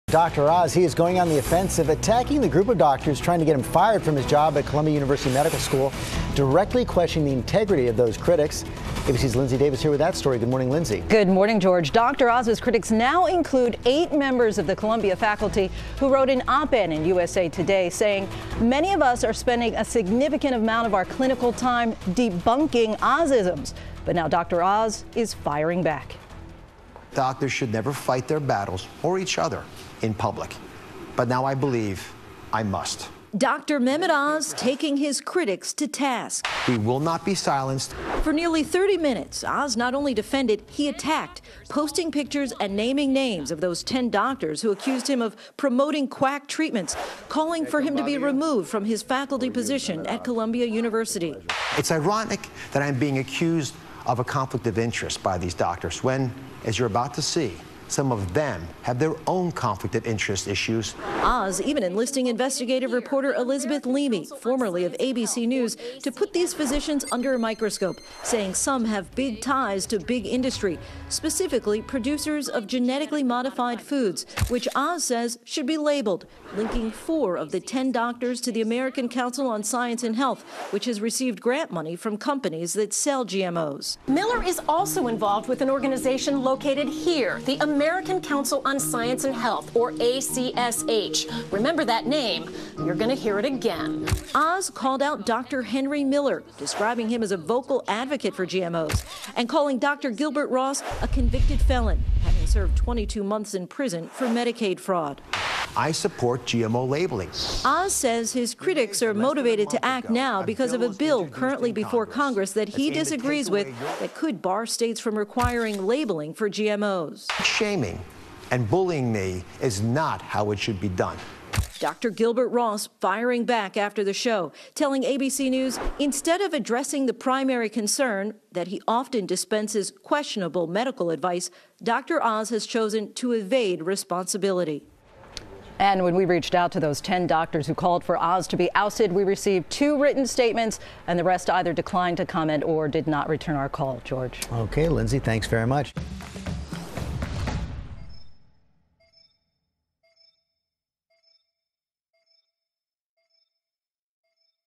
Dr. Oz Fights Back Against Critics
The TV doctor fires back after 10 doctors wrote an op-ed in USA Today.